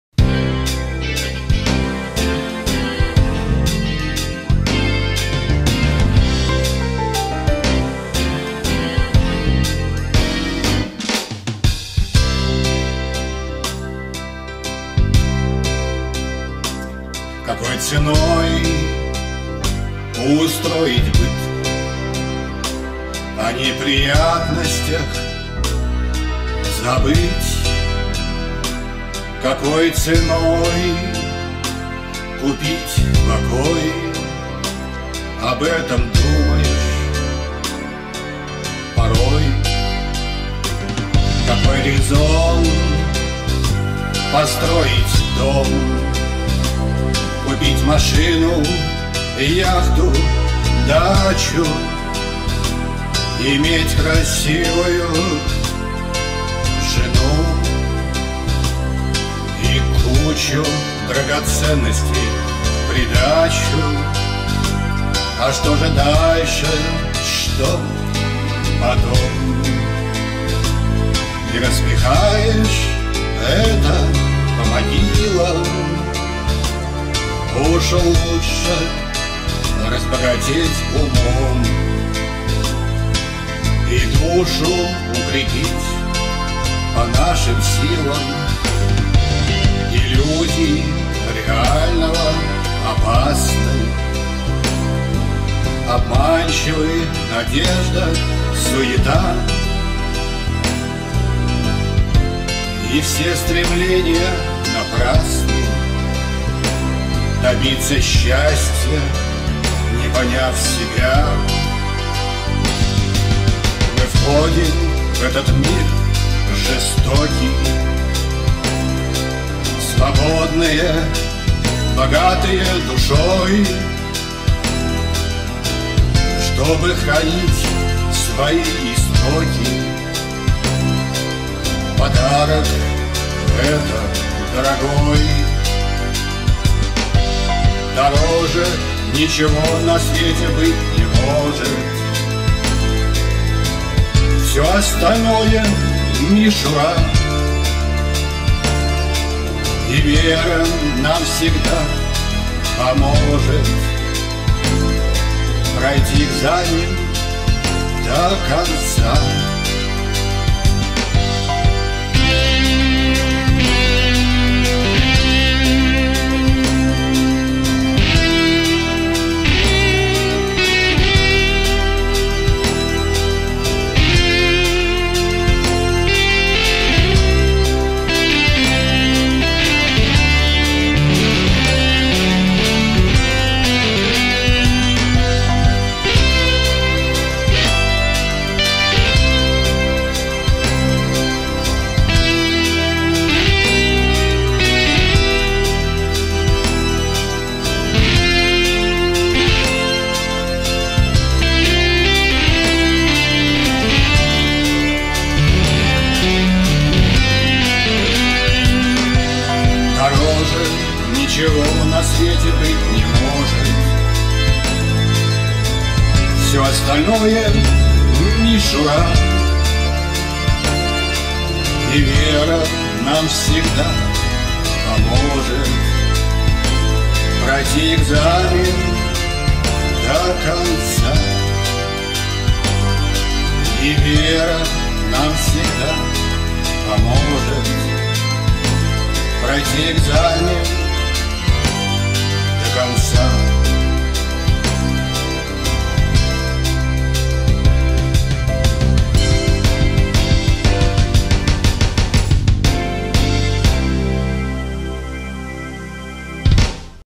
Какой резон построить дом, купить машину, яхту, дачу? - Александр Травин 8 марта 2016
Какой ценой устроить быт.
Видеосъёмка - Игорь Стесев и Вадим Плюско.
Россия - Санкт-Петербург.
Videography - Igor Stesev and Vadim Plusko.
Russia, Saint-Petersburg.